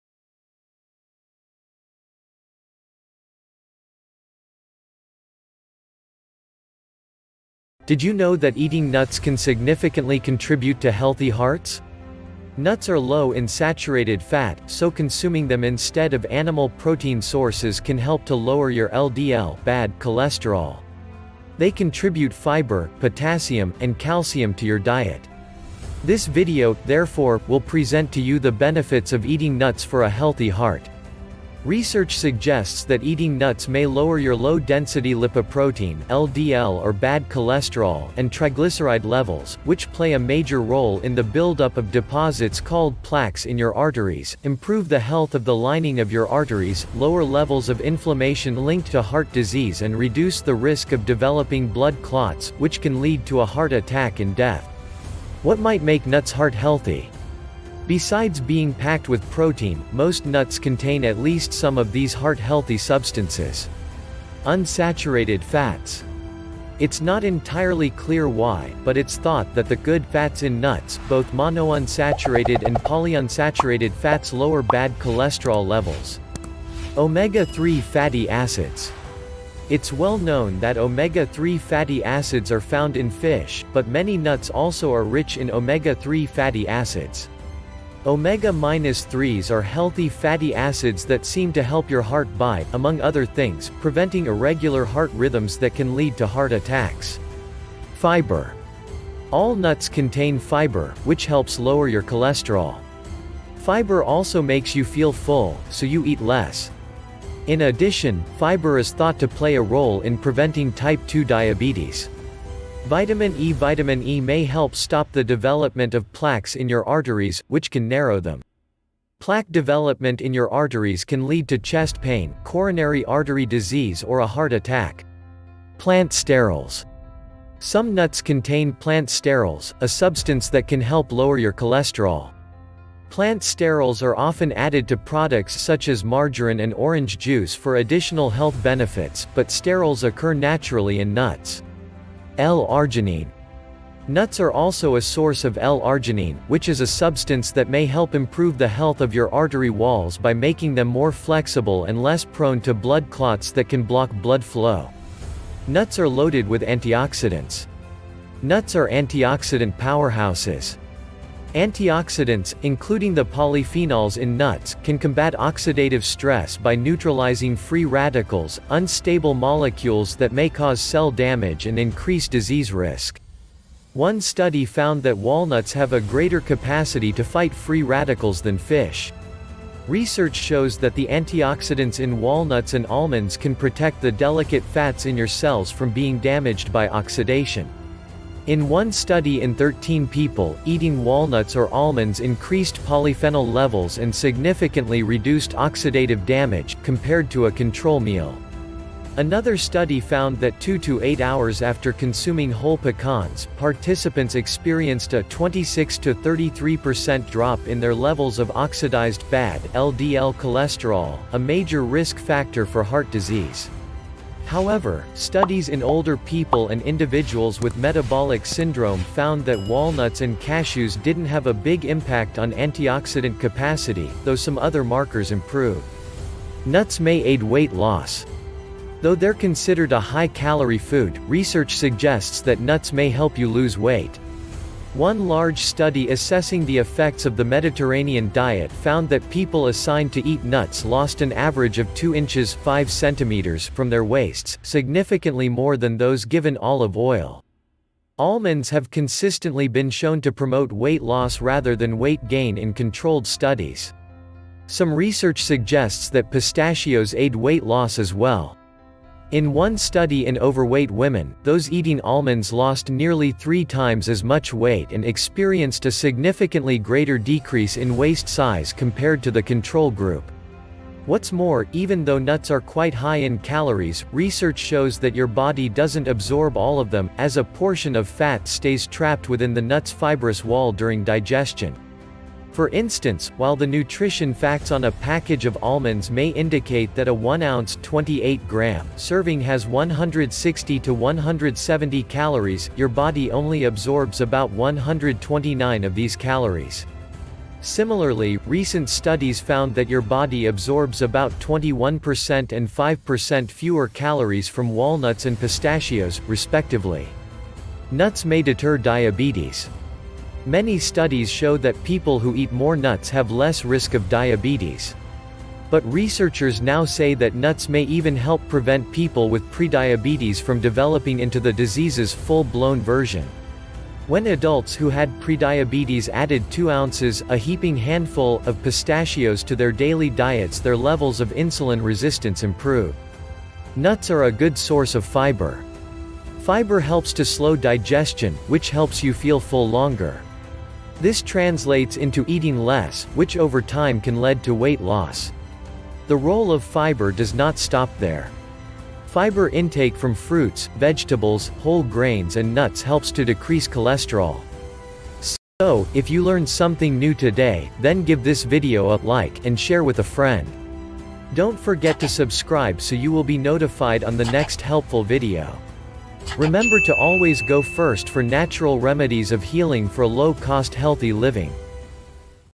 Proven Effective Arthritis Exercises
Did you know that arthritis exercises are beneficial for relieving pain and joint stiffness?  Arthritis exercises and stretching can be particularly helpful during a flare. People with arthritis who exercise may find that they have less pain than those who do not.  Exercise can reduce painful symptoms, improve joint function and flexibility, increase range of motion and boost mood. This video will showcase the proven effective arthritis exercises for you to learn more.